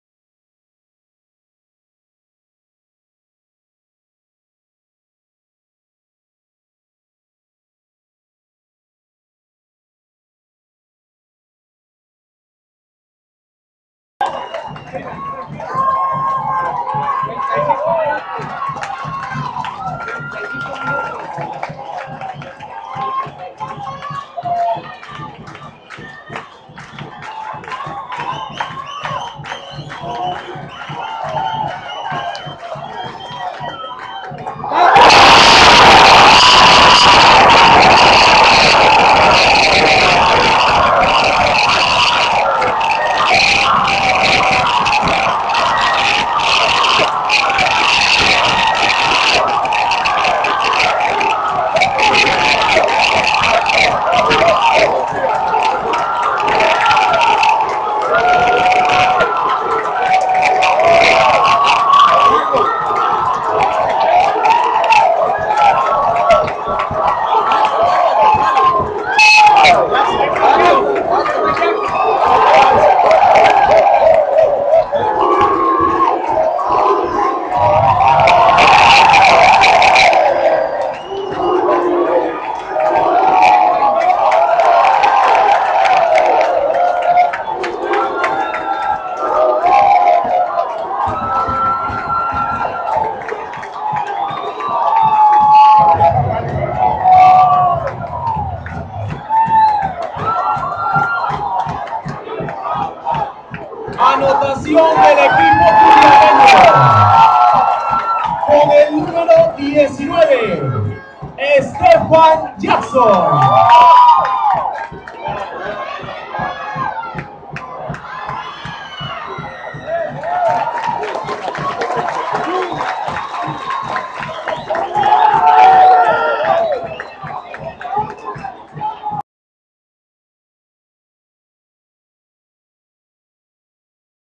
Video del Gool contra Barrio Mexico 20 de Diciembre 2009 lista.wmv
Turrialba recibio de vista a Barrio Mexico en uno de los dos partidos  finales para el torneo de apertura en la liga de asenso ganando 1 a 0 el 20 de Diciembre del 2009 ,siga paso a paso el encuentro de Vuelta este 26 de diciembre desde  El Coyella Fonseca aqui en TurrialbaDigital.com